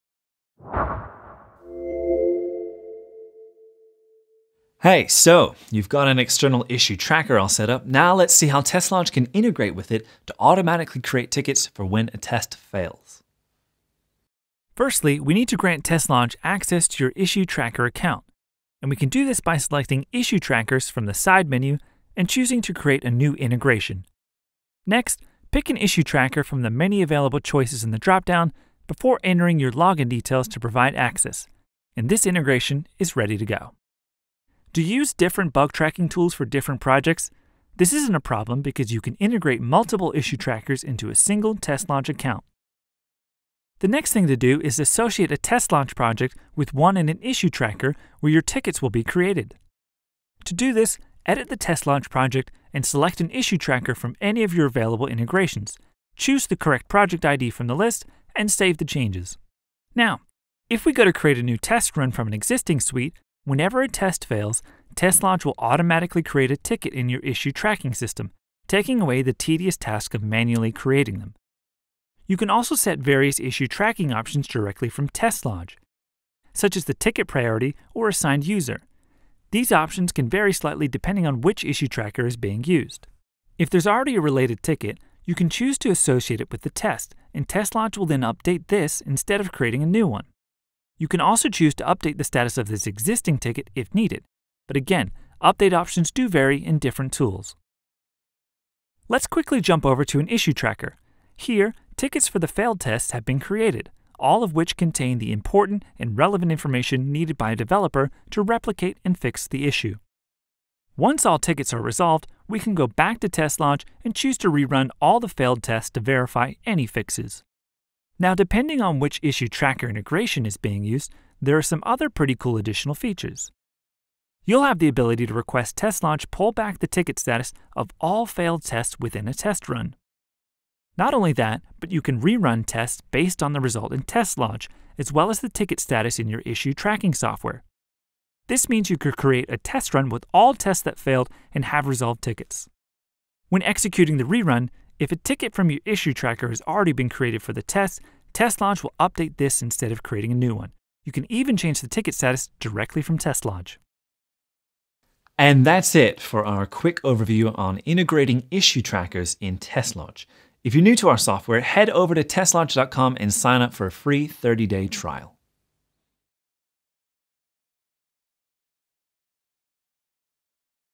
Issue Tracker Integration – TestLodge Tutorials
A quick overview of how TestLodge integrates with issue trackers so tickets can be created automatically whenever a test fails.

For further reading on integrations, take a look at this article

0:00 - Intro
0:14 - How to setup an integration
0:43 - Associate your projects
1:00 - Automatically raise an issue
1:12 - Set issue options
1:23 - Associate an existing issue
1:38 - View the raised tickets
1:50 - Other functionality
2:04 - Sync back issue status
2:10 - Re-run tests and verify fixes
2:23 - Update and resolve issues
2:35 - Outro